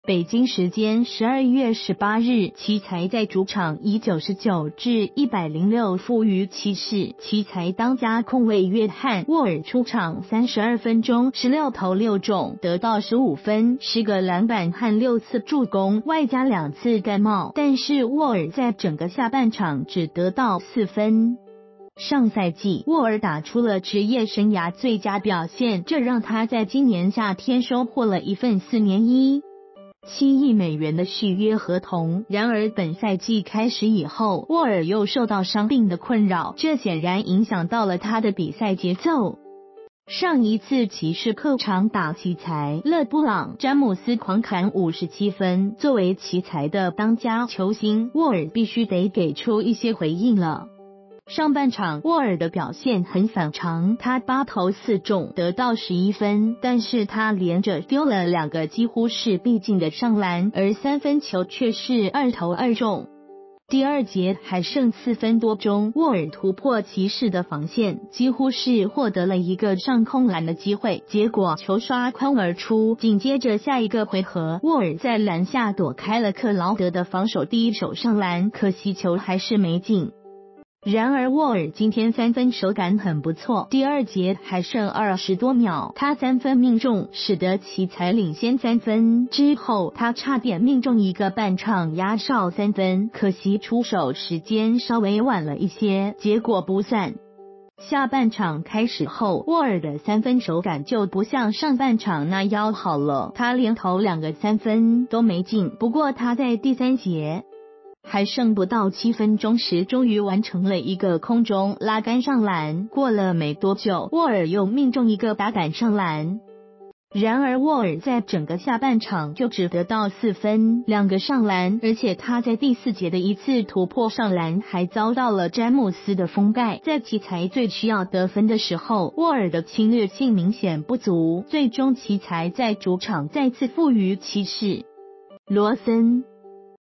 下半场只4分！东部首席PG被詹姆斯1帽扇懵了？
發表評論評論如下。